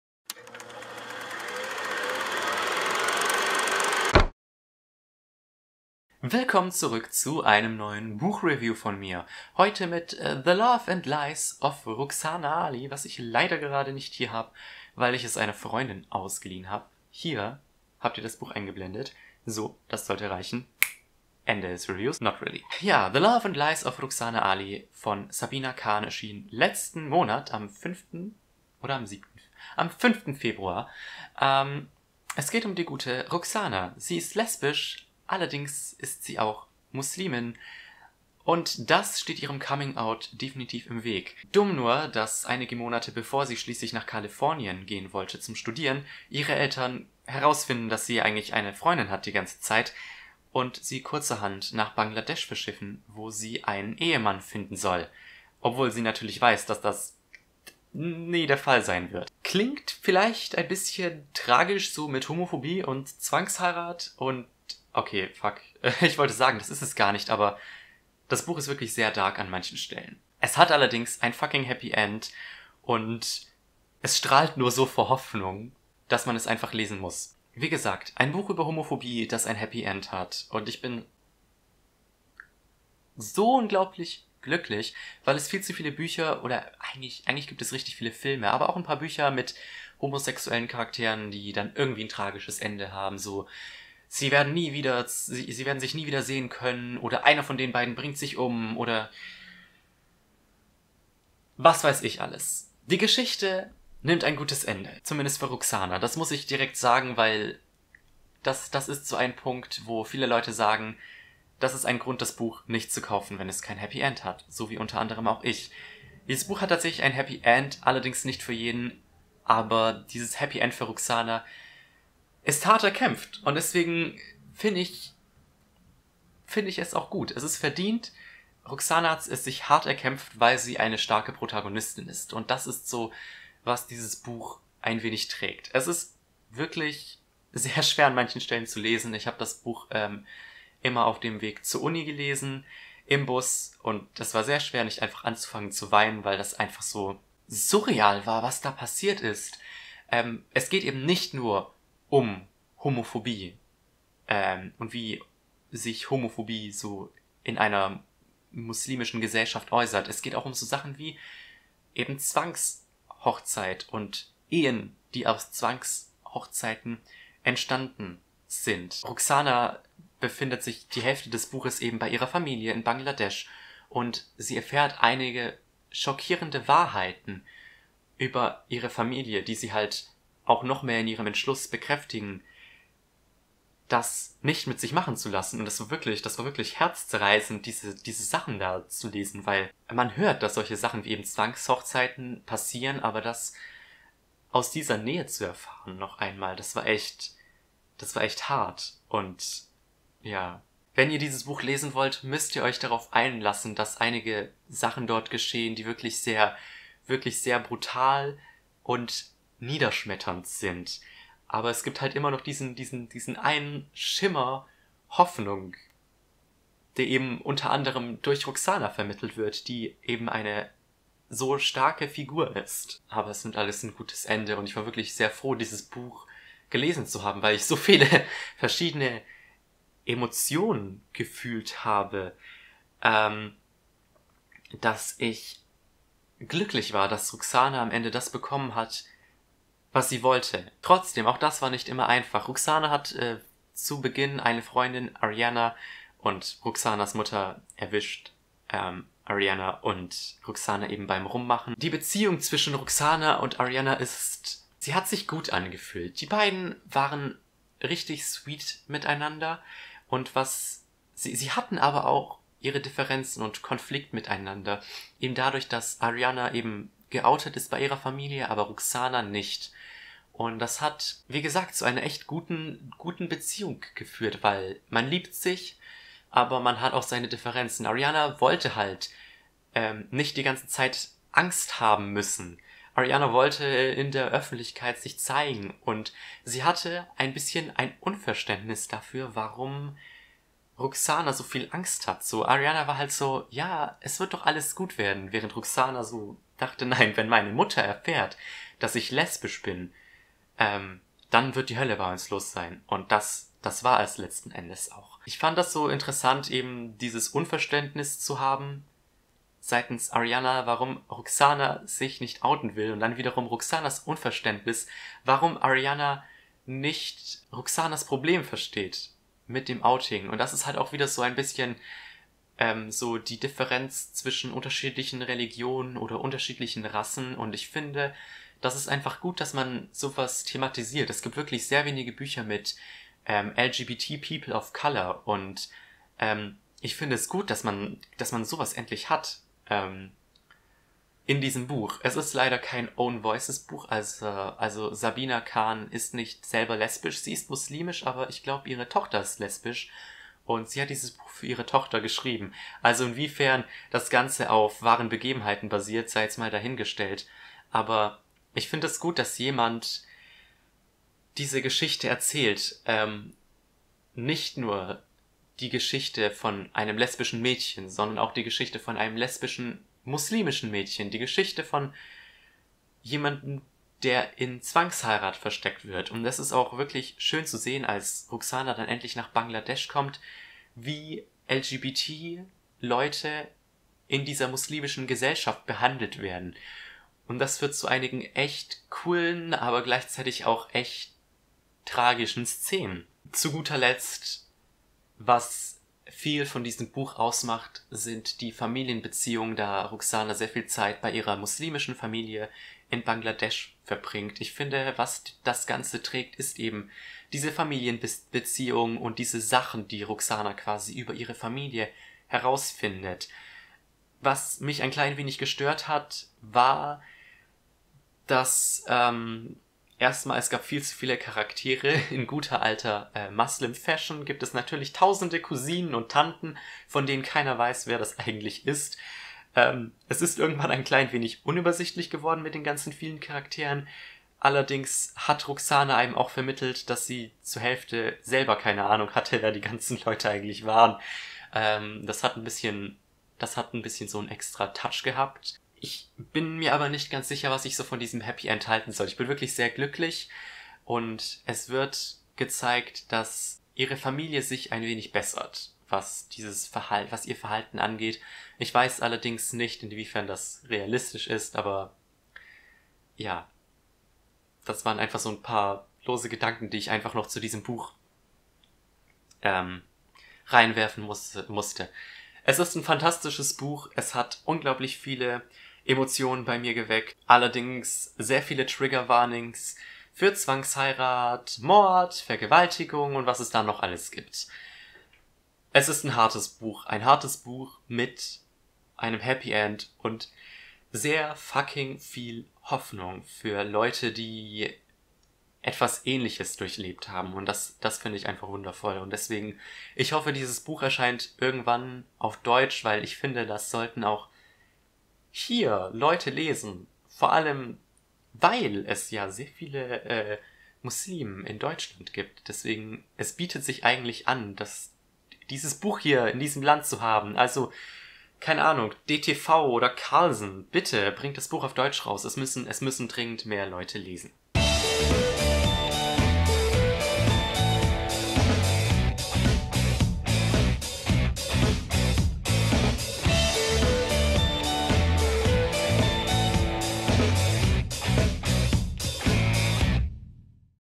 The Love & Lies of Rukhsana Ali | Buch Review
Ein Coming Out Buch, dass man definitiv gelesen haben sollte.